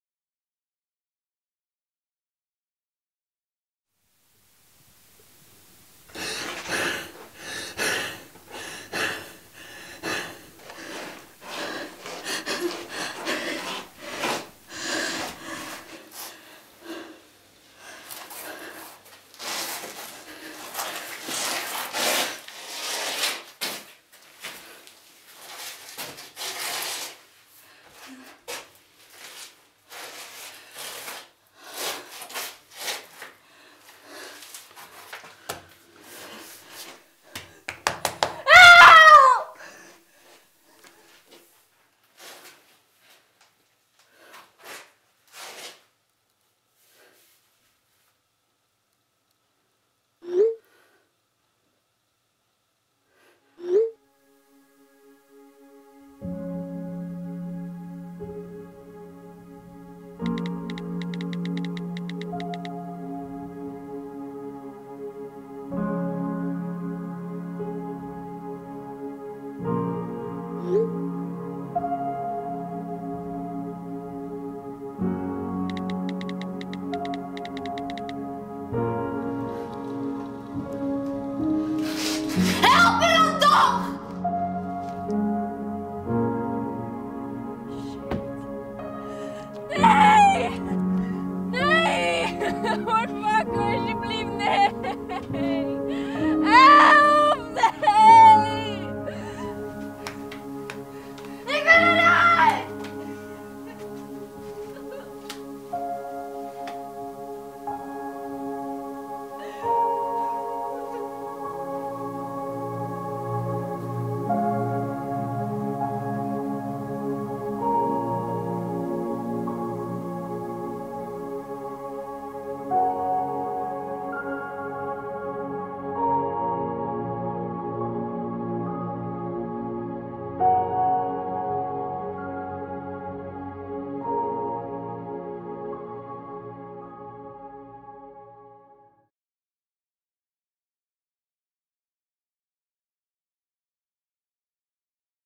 ERUIT
Een korte film die ik voor school heb gemaakt.

MET DANK AAN:
Veerle de Jong
Anne Visser
Hugo Drechsler
Renzo Westerbeek
Albert Klein Haneveld
Laura Bromet
Frans Bromet